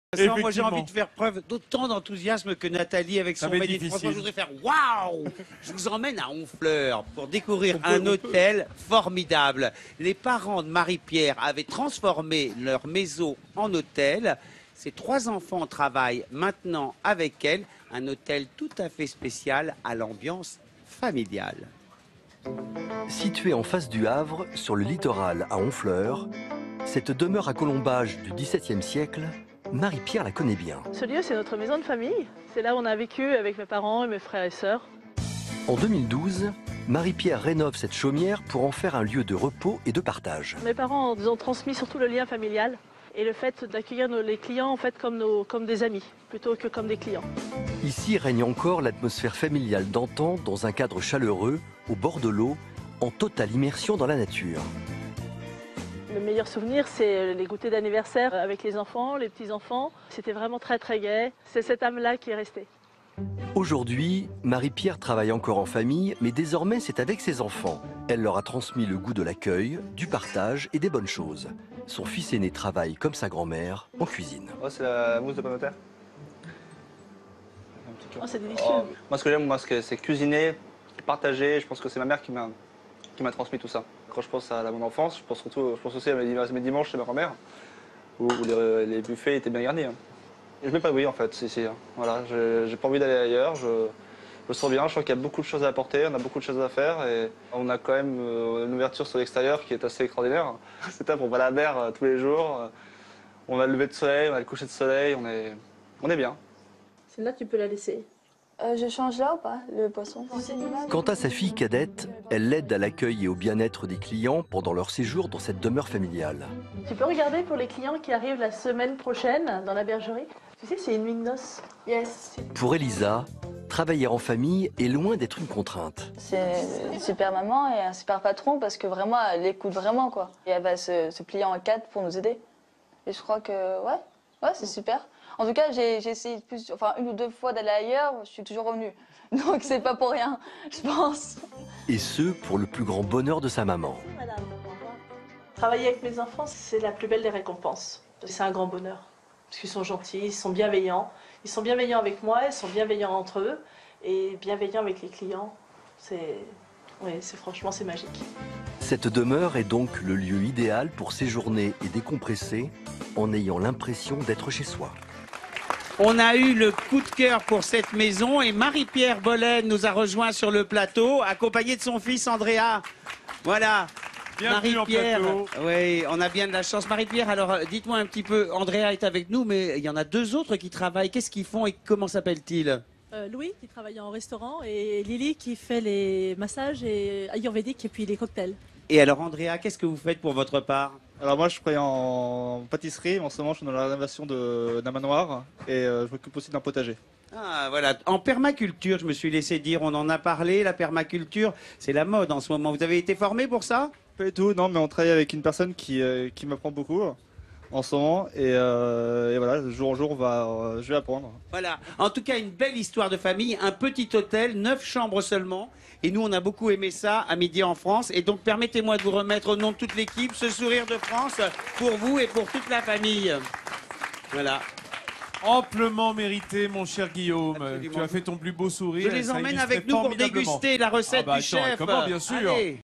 Une chaleureuse chaumière normande au bord de l'estuaire
Située en face du Havre, sur le littoral, à Honfleur, cette demeure à colombages du 17ème siècle, Marie-Pier la connait bien. En 2012, Marie Pier rénove cette chaumière pour en faire un lieu de repos et de partage. Ici règne encore l’atmosphère familiale d’antan dans un cadre chaleureux, au bord de l’eau, en totale immersion dans la nature. Aujourd’hui, Marie Pier travaille encore en famille mais désormais c’est avec ses enfants. Elle a leur transmis le goût de l’accueil, du partage et des bonnes choses. Son fils aîné  travaille, comme sa grand-mère, en cuisine. Sa fille cadette, elle, l’aide à l’accueil et au bien-être des clients pendant leur séjour dans cette demeure familiale. Pour Elisa, travailler en famille est loin d’être une contrainte ! Et ce, pour le plus grand bonheur de sa maman ! Cette demeure est donc le lieu idéal pour séjourner et décompresser en ayant l’impression d’être chez soi!